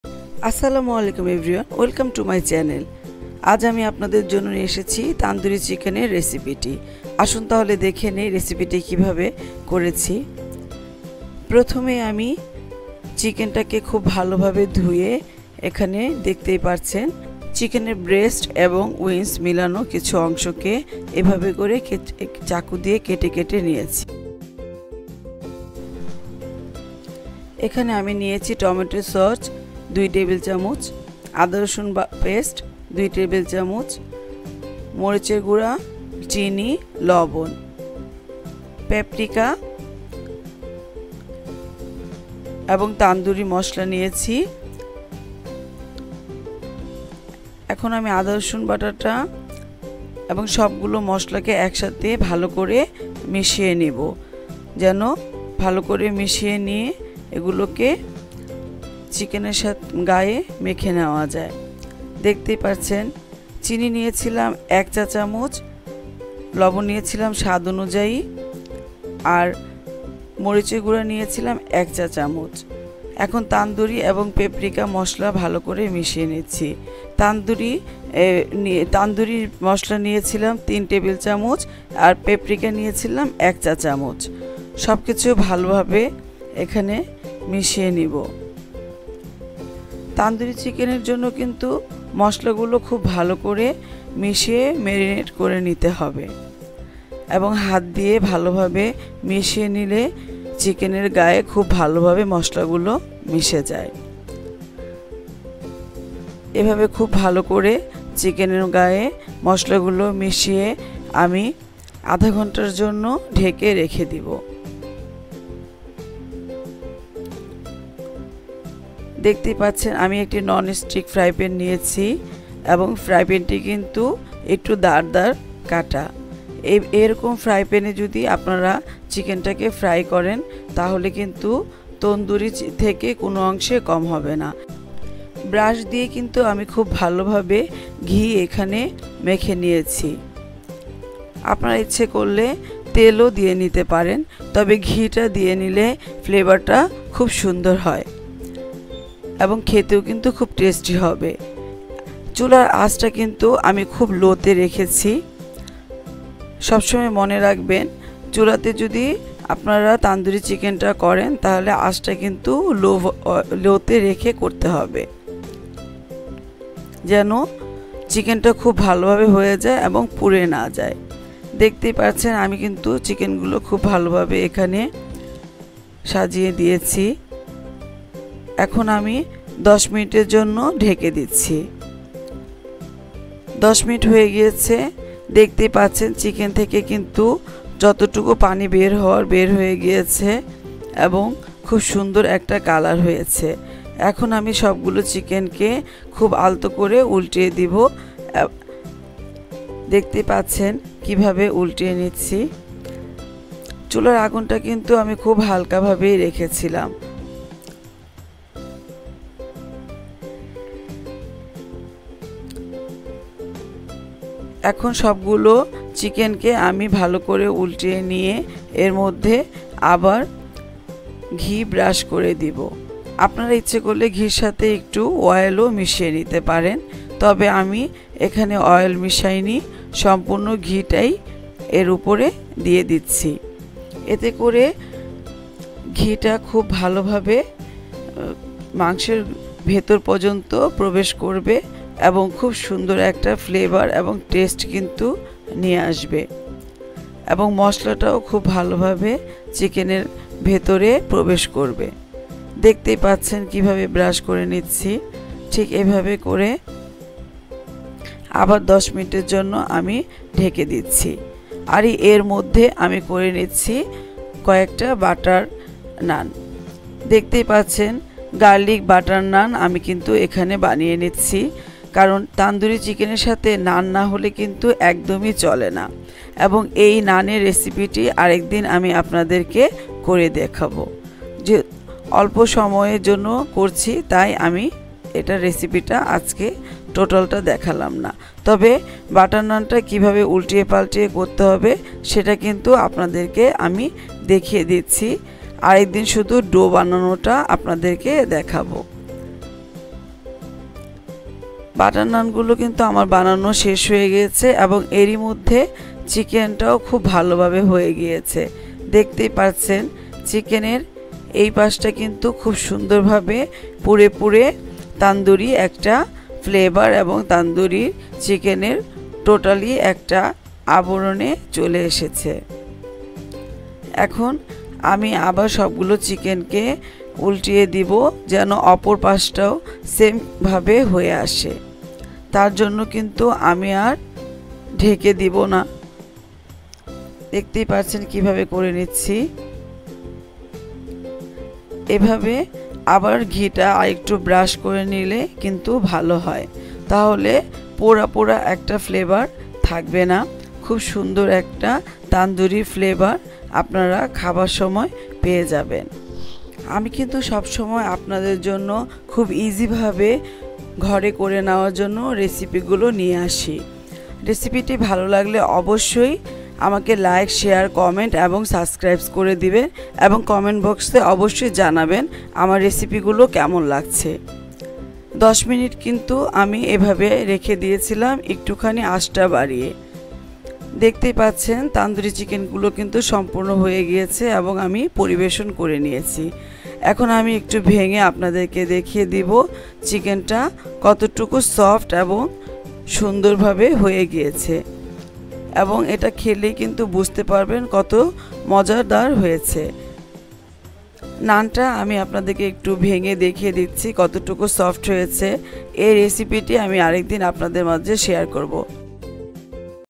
Tandoori chicken with garlic butter naan easy recipe😊👆
Ingredients_
# chicken wins pieces
# tomato sauce 2 table spoon
# garlic ginger paste 2 tablespoon
# sugar 1 teaspoon
# chilli powder one teaspoon
# salt one teaspoon
# Paprika powder one teaspoon
# Tandoori mixed masala 2 table spoon
# gee😊